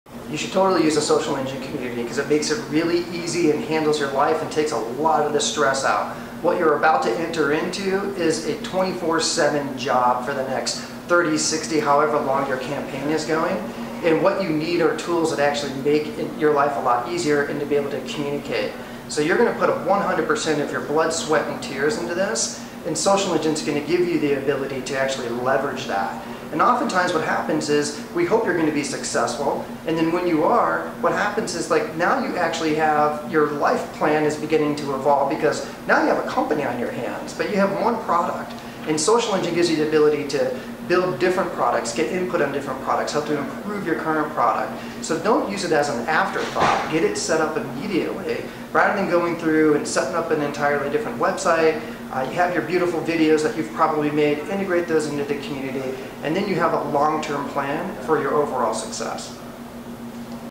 Online Community and Crowdfunding - Make the Connection
Learn why there are inherent benefits to creating and connecting an online community with your crowdfunding campaign.